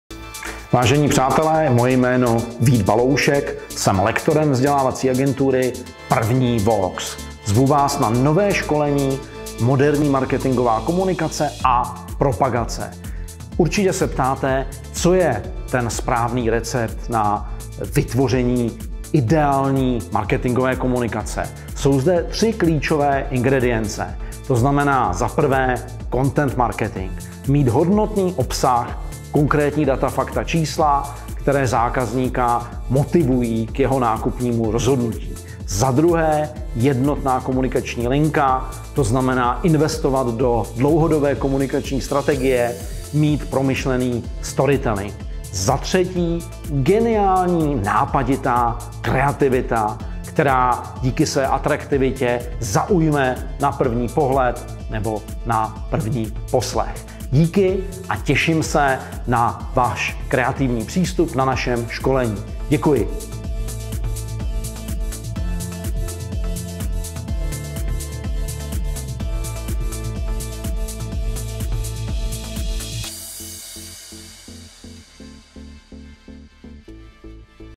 Moderní marketingová komunikace | Kurzy VOX
K úspěchu budete potřebovat ✅

👉 Výstižný text
👉 Který je součástí komunikační strategie
👉 S dávkou kreativity
👉 Sdílený správným komunikačním kanálem
👉 Ve spolupráci se správnou marketingovou agenturou (ale jaká to je?)

Co každý z uvedených bodů obnáší, to se dozvíte na semináři marketingového odborníka Víta Balouška

Zákazníky nezajímá, co chcete prodat ❌ Zákazníky zajímá pouze to, co chtějí koupit ✅ Jak to ale podat, aby vše pochopili❓ Zdánlivě jednoduchá otázka nemá vždy jednoduché řešení a zejména ne ve chvíli, kdy je reklam plný internet a všechny se přetahují o pozornost zákazníka 🤯